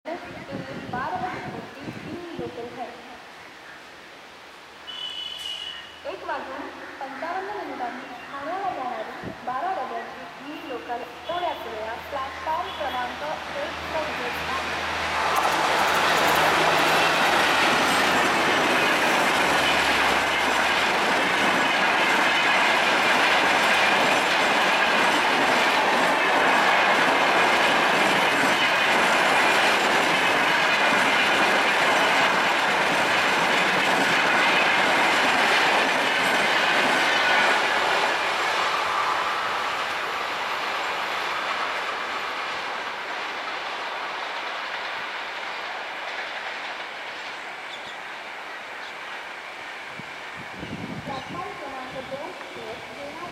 RECENTLY AUGMENTED CR's RAJDHANI EXPRESS TAKEN AT BHANDUP ON SUMMER AFTERNOON
22110 Hazrat Nizamuddin Lokmanya Tilak Terminus AC Express behind BSL WAP-4# 22929 speeds past Bhandup station in Mumbai on a hot May afternoon.

22109/22110 Lokmanya Tilak Terminal Hazrat Nizamuddin- Lokmanya Tilak Terminal A.C Express train has been augmented by two additional A.C. 2 Tier coach from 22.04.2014 to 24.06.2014 when departing from Lokmanya Tilak Terminal and from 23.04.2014 to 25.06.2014 when departing from Hazrat Nizamuddin. This train runs full since Oct 2013, and I am sure that it will be a bigger hit and more coaches will be added too in future.